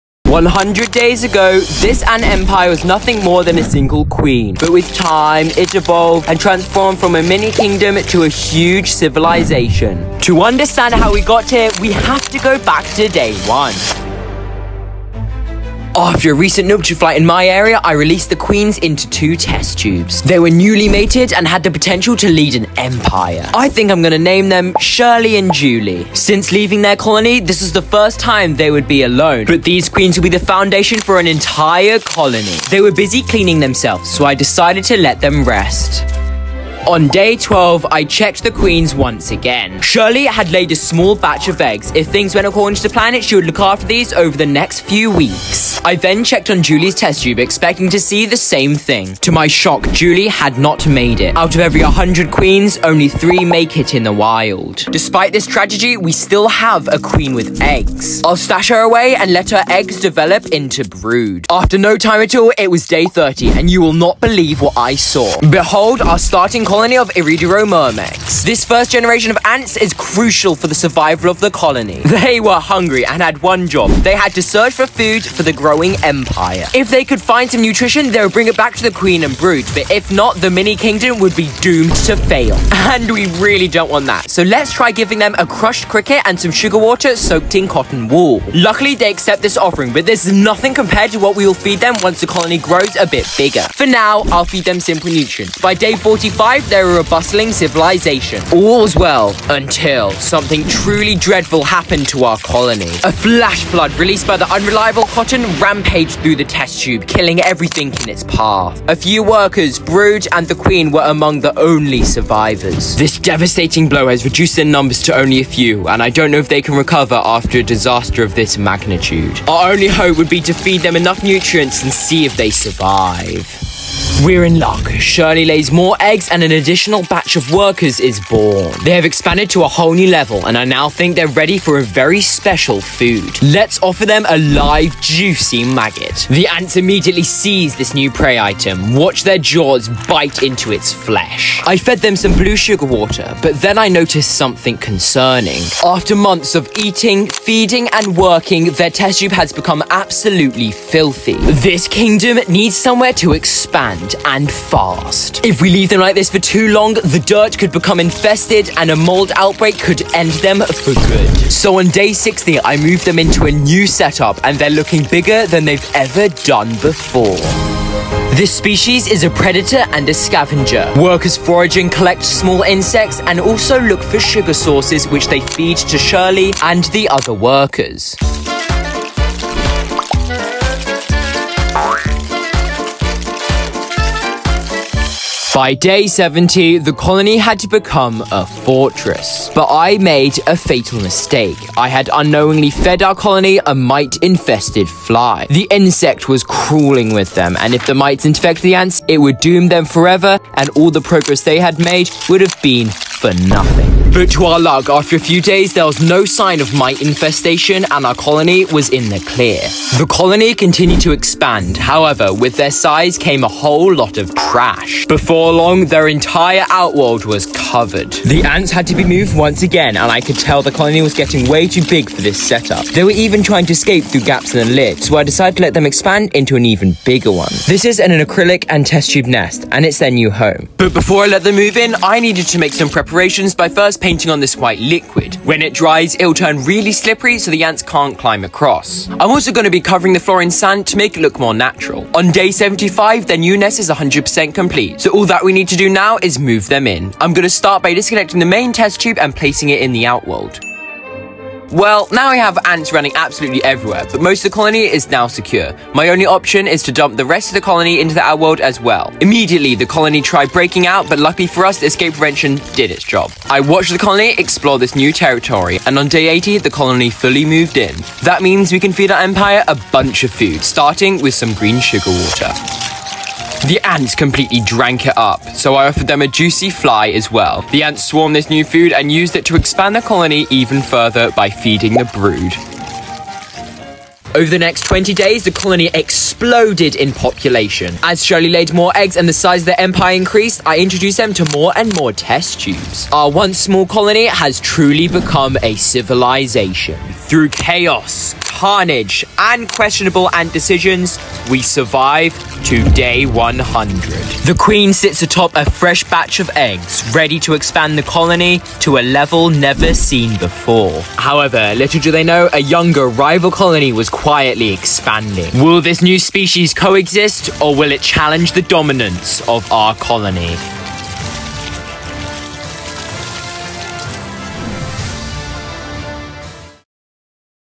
Simulating an Ant Colony For 100 Days
In this video I got a queen rainbow ant (Iridromyrmex sp.) and raised it into an entire ant colony! Throughout the video the colony faces several challenges as they grow. The ending is shocking...